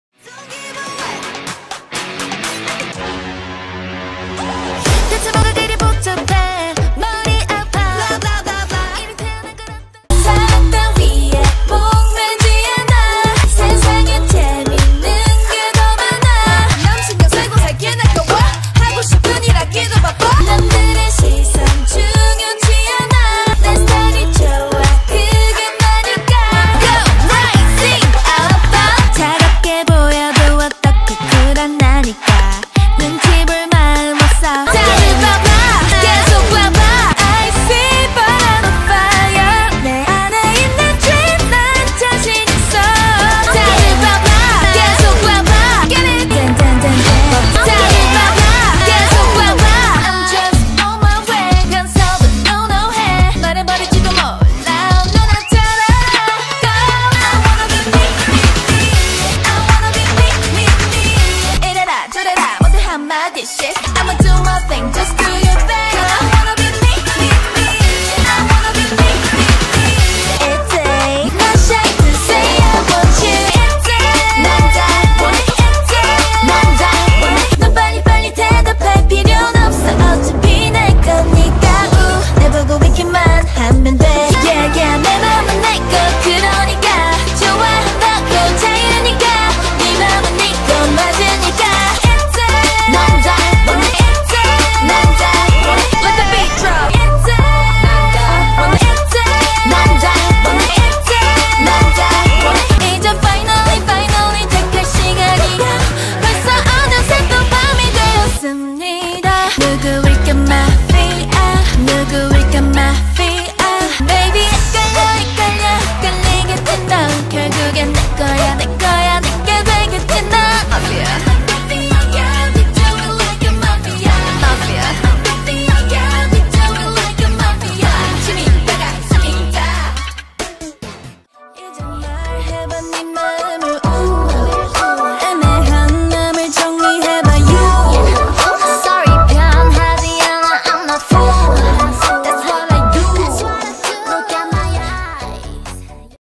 ITZY - Chaeryeong (Line Evolution) • APR/21
Comparison of all Chaeryeong's amount of lines on the korean title tracks.